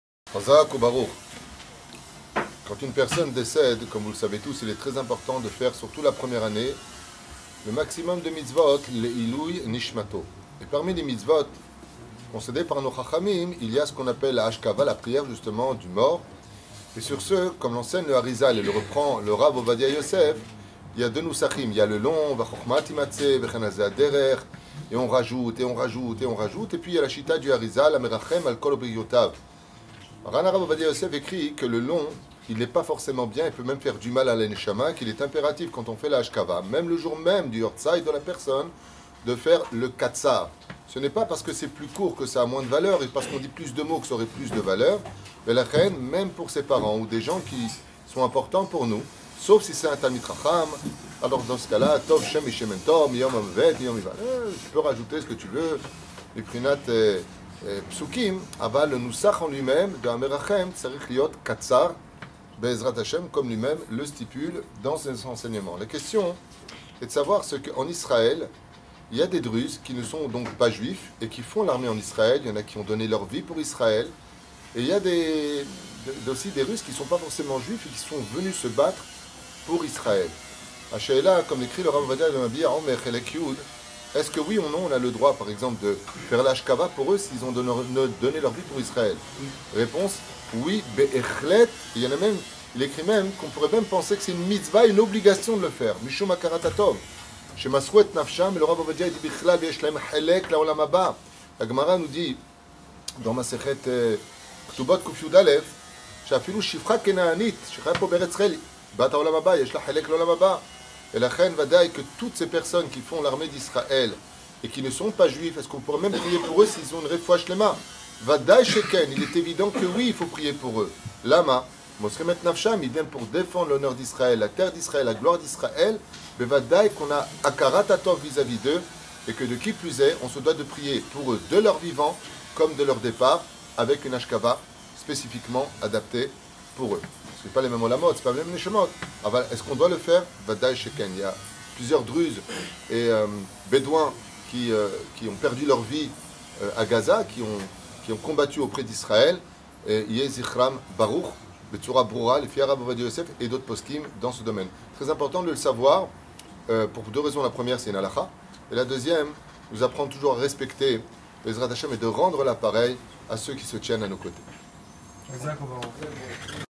Ashkava et prières pour les non-juifs qui combattent auprès d’Israël 🇮🇱
2 Halakhot pour être ben olam aba 🔥
 Retrouvez tous les cours sur Torathaim.net